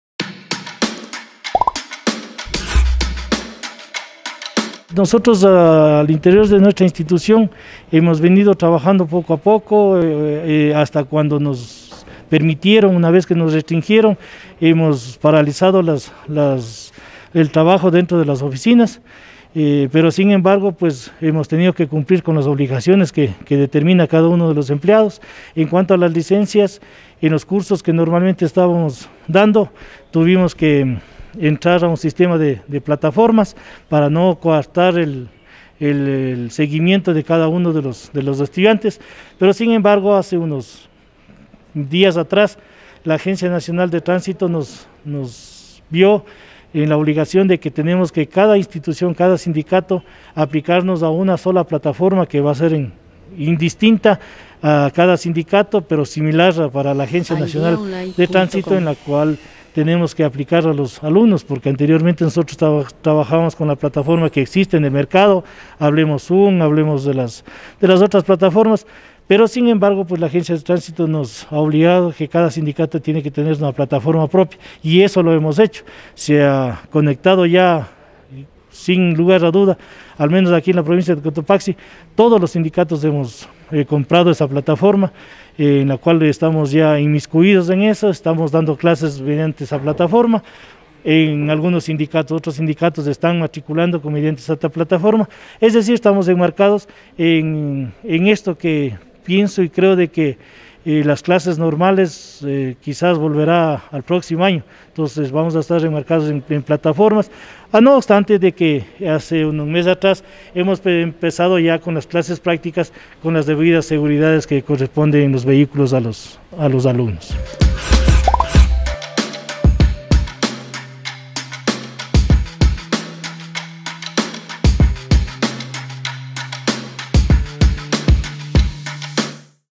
Iván Herrera Sec. General del Sindicato de Choferes de Pujilí informa de cursos de capacitación
Iván Herrera Secretario General del Sindicato de Choferes de Pujilí informa sobre los cursos de capacitación